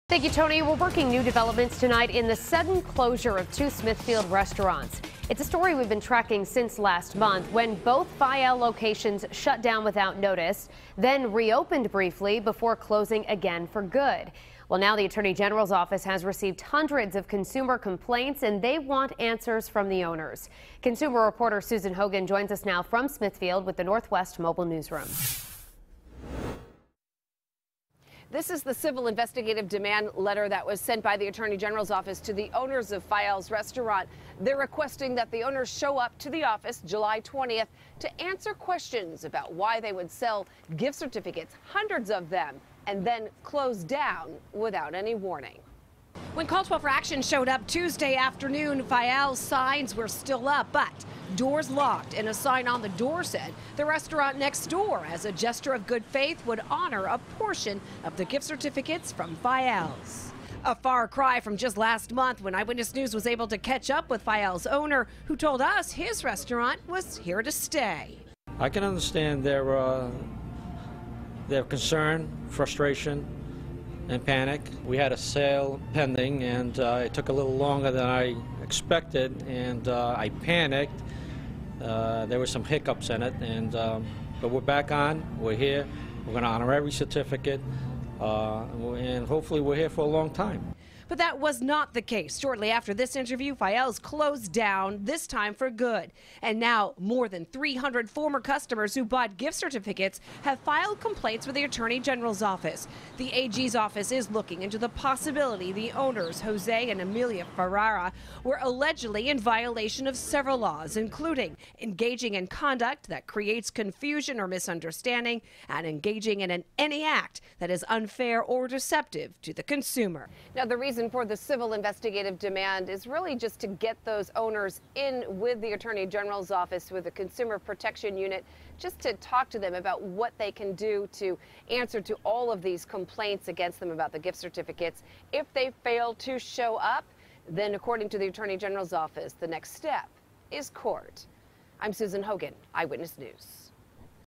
Closed Restaurants' Owners Ordered Before Attorney General
Attorney General Peter Kilmartin is ordering the owners of a pair of closed restaurants to appear after multiple complaints from customers.